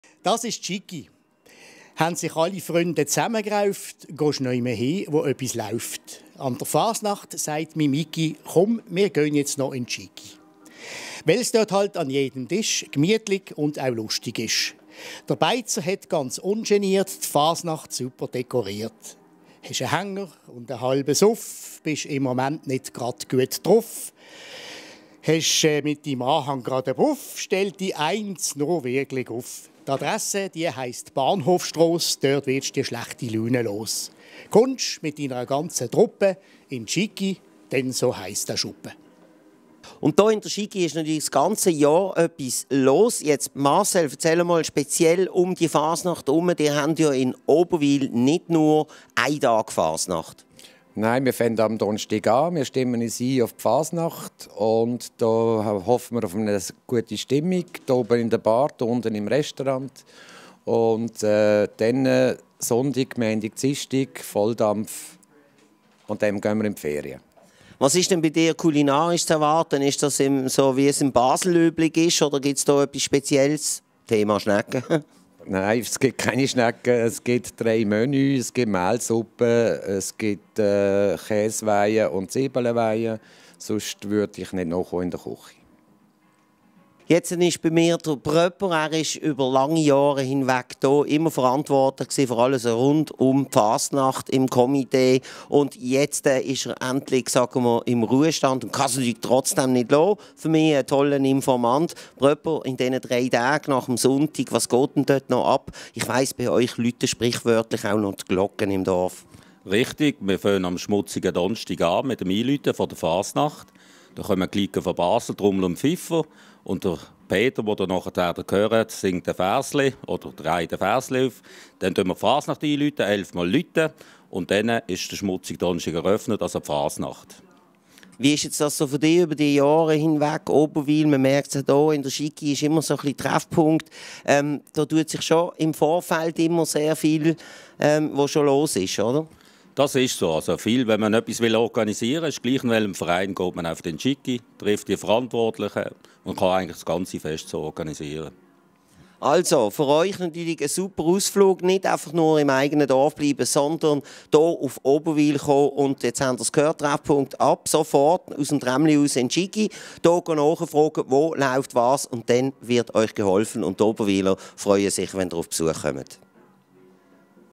Treffpunkt Schikeria
Fasnacht in Oberwil, jetzt geht es los…
regioTVplus 17-KW-07